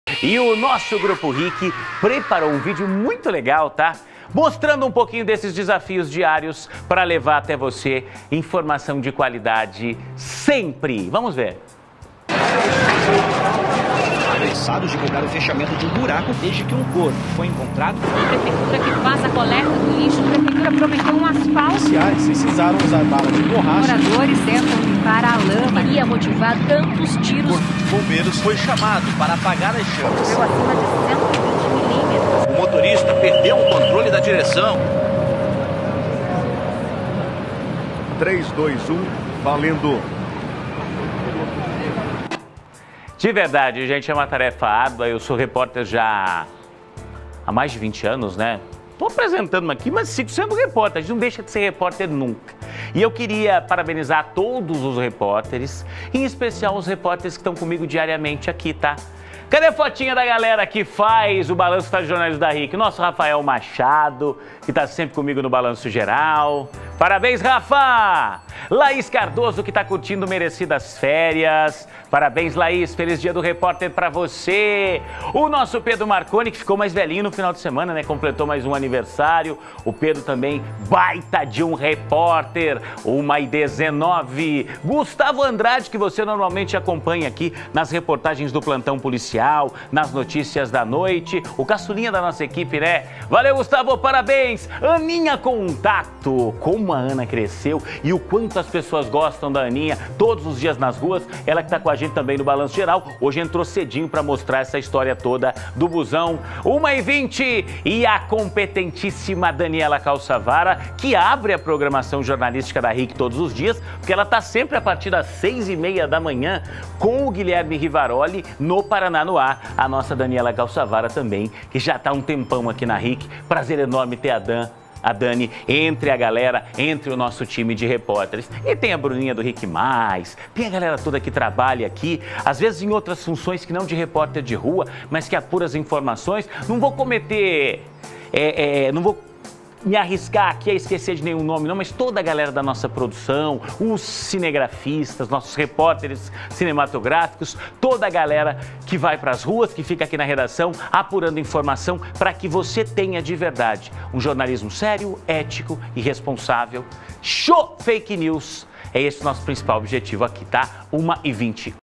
No dia do repórter, BG homenageia os que te levam informação todos os dias
E você sabia que hoje é comemorado o dia do repórter? pois é!
E é claro que a gente fez questão de celebrar com os repórteres aqui da Ric!